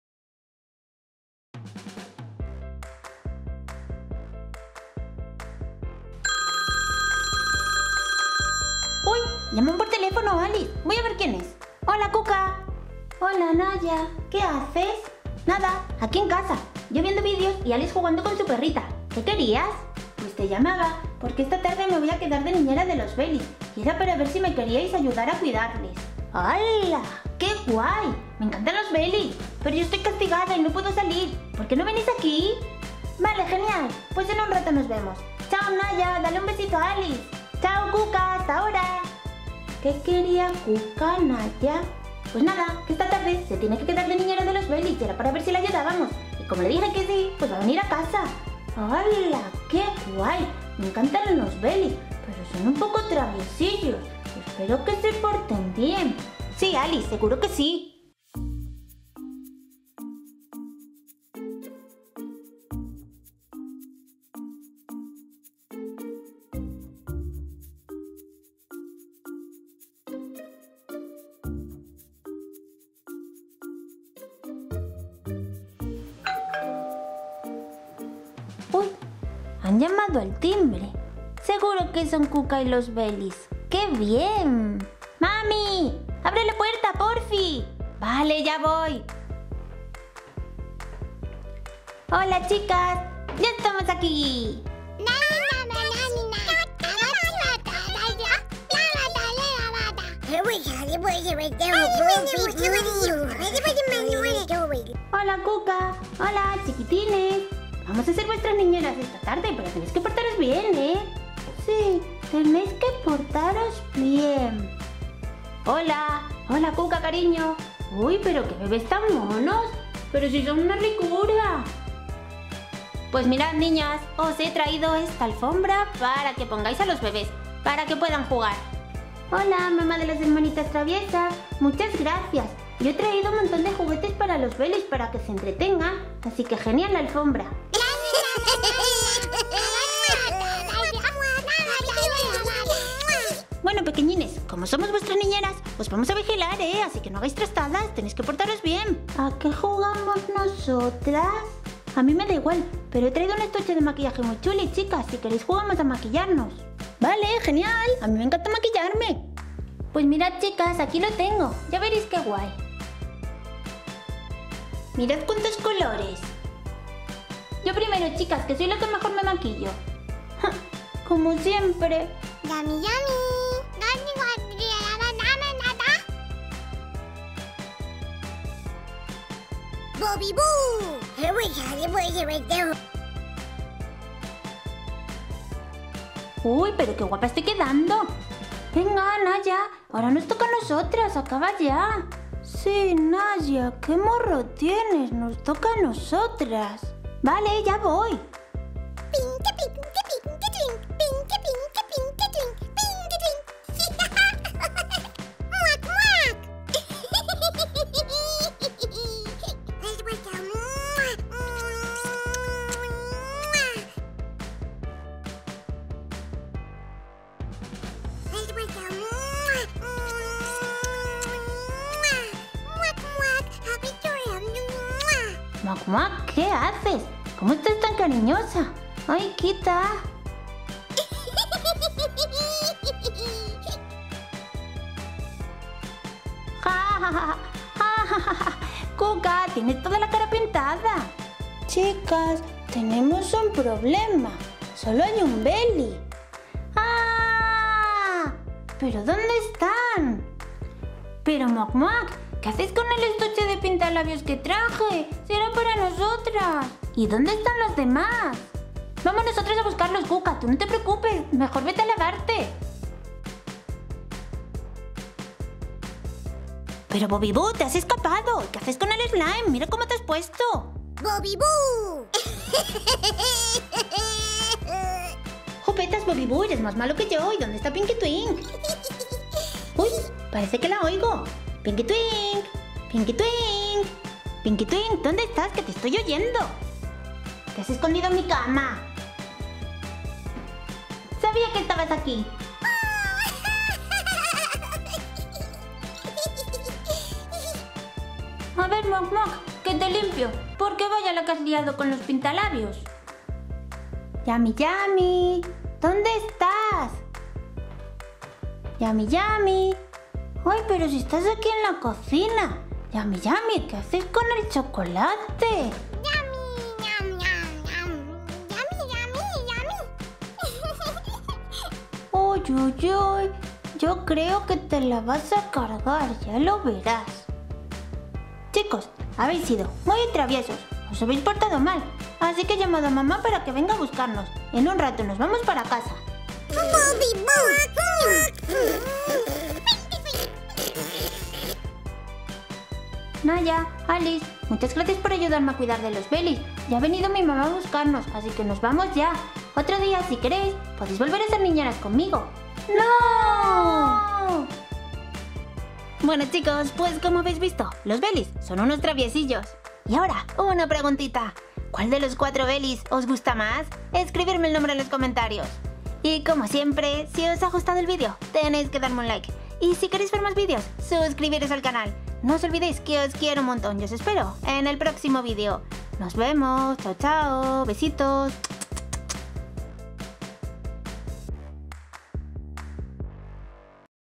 Bebés Nenuco NAIA y ALICE Niñeras por un día de los Bebés BELLIES
¡Hola chicos! Las Hermanitas Traviesas Naia y Alice, van a ayudar a la Nenuco Princesa Cuca a ser niñera, y tendrán que cuidar de los Bebés Bellies. Y a estos Bebés, les encanta hacer travesuras.
Puedes ver más Vídeos de Juguetes y Aventuras de Bebés Nenuco, Ksi Meritos e Historias con muñecas Bebés Baby Born, para niños y niñas en el Canal!